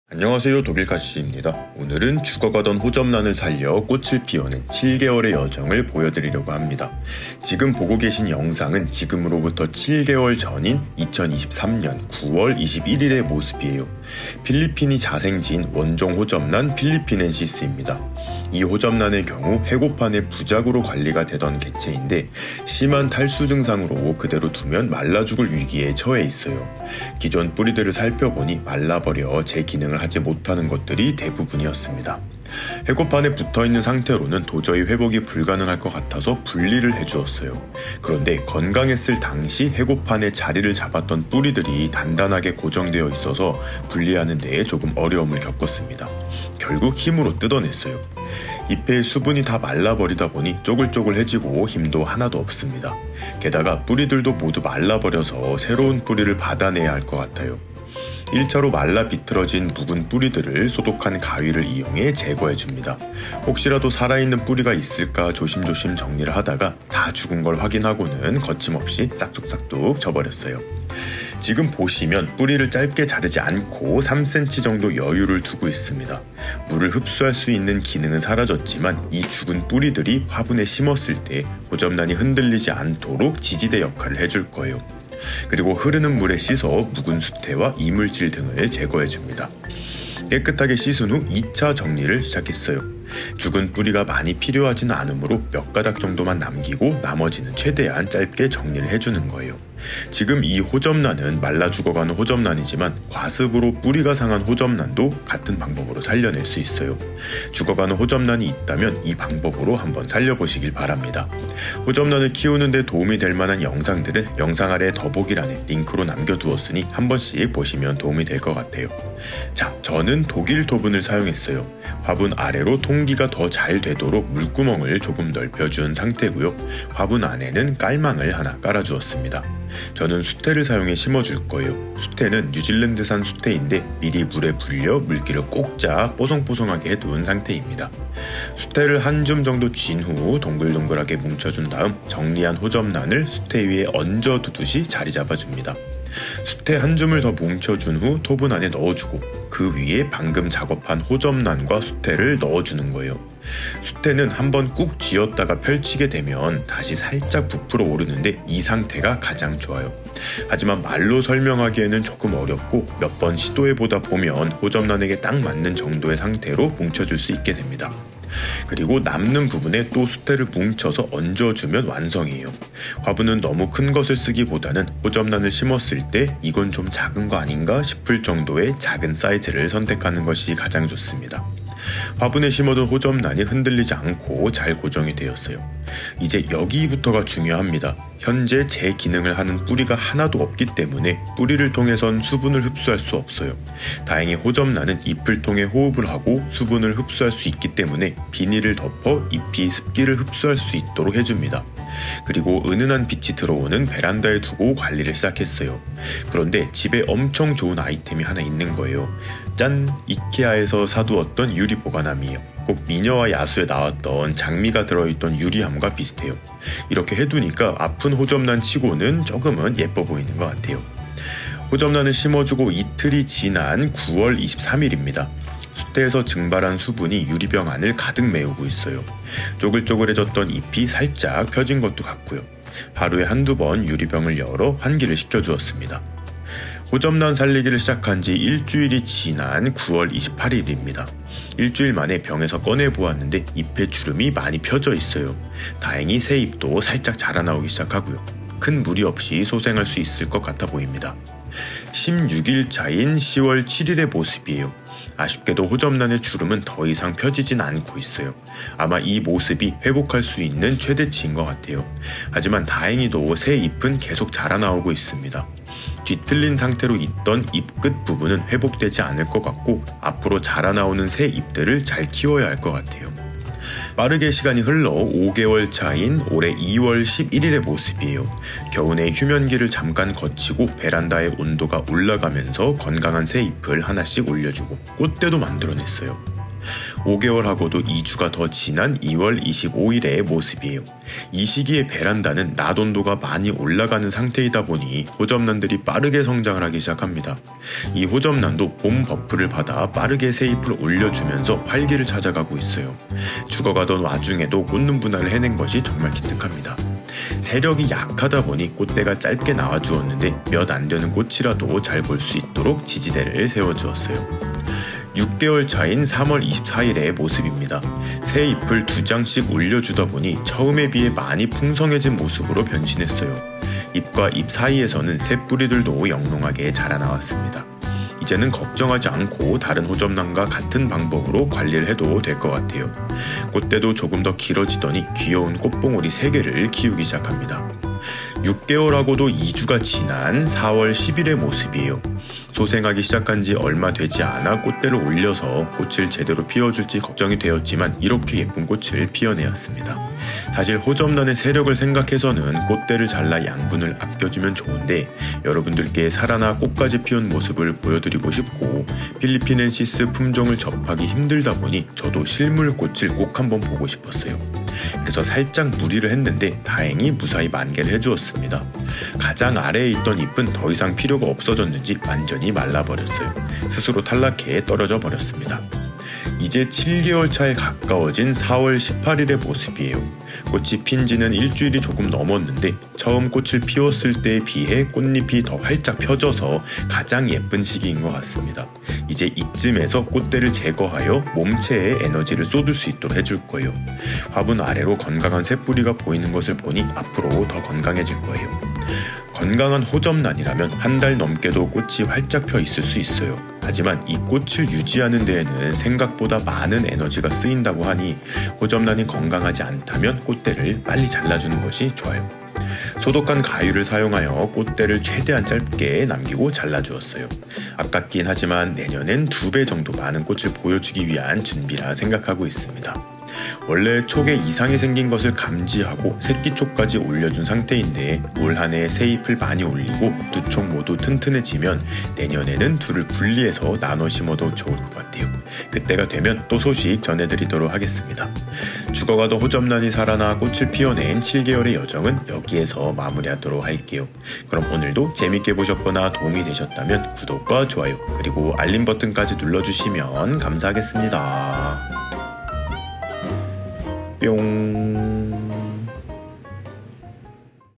다 죽은 호접란 데려와 꽃 피웠어요!! 호접란 살리는 방법과 7개월의 여정!!
두 번째 식물책 "식물을 배우는 시간" 온라인 서점 링크